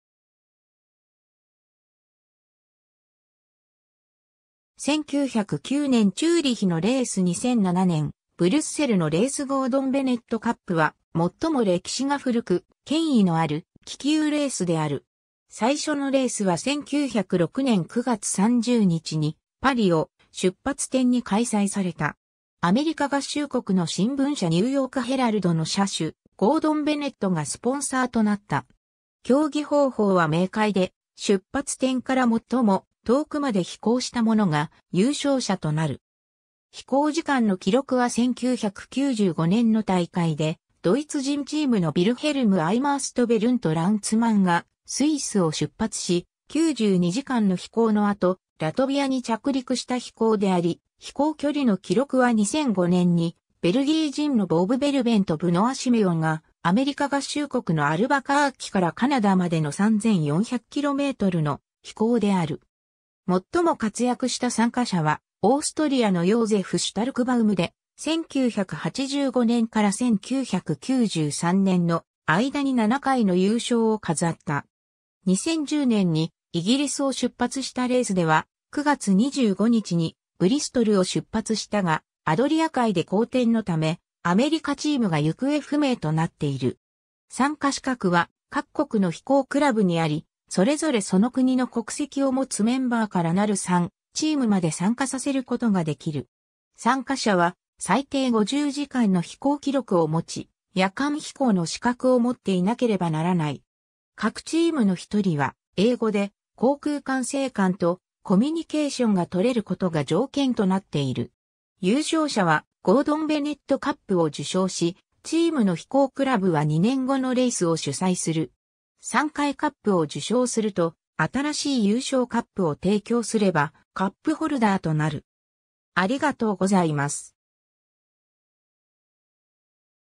ゴードン・ベネット・カップ (気球レース)
1909年チューリヒのレース 2007年ブリュッセルのレース ゴードン・ベネット・カップ（英語: Gordon Bennett Cup、フランス語: Coupe Aéronautique Gordon Bennett）は、最も歴史が古く、権威のある気球レースである。
最初のレースは1906年9月30日に、パリを出発点に開催された。
アメリカ合衆国の新聞社ニューヨーク・ヘラルドの社主、ゴードン・ベネットがスポンサーとなった。
競技方法は明快で、出発点から最も遠くまで飛行した者が優勝者となる。
 飛行時間の記録は1995年の大会で、ドイツ人チームのヴィルヘルム・アイマース（Wilhelm Eimers）とベルント・ランツマン（Bernd Landsmann）がスイスを出発し、92時間の飛行の後、ラトビアに着陸した飛行であり、飛行距離の記録は2005年にベルギー人のボブ・ベルベン（Bob Berben）とブノア・シメオン（Benoît Siméons）がアメリカ合衆国のアルバカーキからカナダまでの3,400kmの飛行である。
最も活躍した参加者は、オーストリアのヨーゼフ・シュタルクバウム（Josef Starkbaum）で、1985年から1993年の間に7回の優勝を飾った。
 2010年にイギリスを出発したレースでは、9月25日にブリストルを出発したが、アドリア海で荒天のため、アメリカチームが行方不明となっている。
 参加資格は各国の飛行クラブにあり、それぞれその国の国籍を持つメンバーからなる3チームまで参加させることができる。
参加者は最低50時間の飛行記録を持ち、夜間飛行の資格を持っていなければならない。
各チームの1人は英語で航空管制官とコミュニケーションが取れることが条件となっている。
優勝者はゴードン・ベネット・カップを受賞し、チームの飛行クラブは2年後のレースを主催する。
3回カップを受賞すると、新しい優勝カップを提供すればカップ・ホルダーとなる。